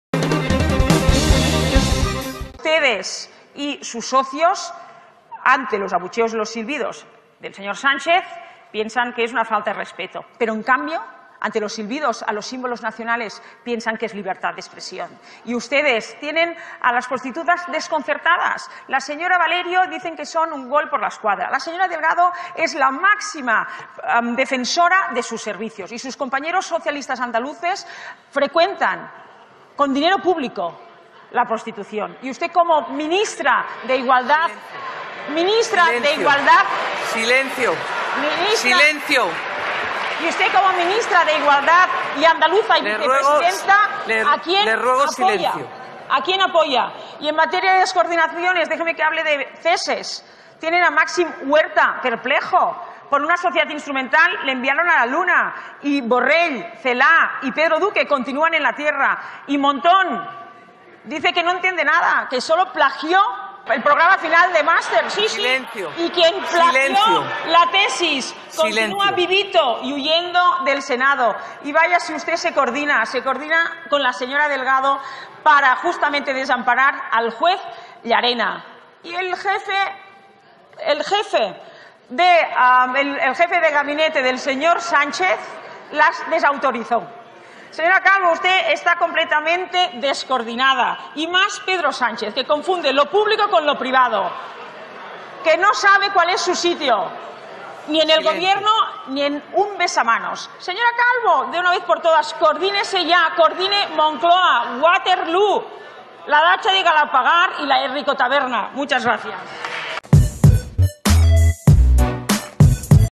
Las arbitrariedades del gobierno Sánchez
Éste canal te ofrece información en sólo un minuto. Todos nuestros vídeos, como máximo, serán de un minuto y fracción.
Dolores Montserrat  da un repaso a la actualidad “íntima” del gobierno okupa con preguntas a la vicepresidenta Calvo : "Waterloo, las prostitutas y la herrikotaberna"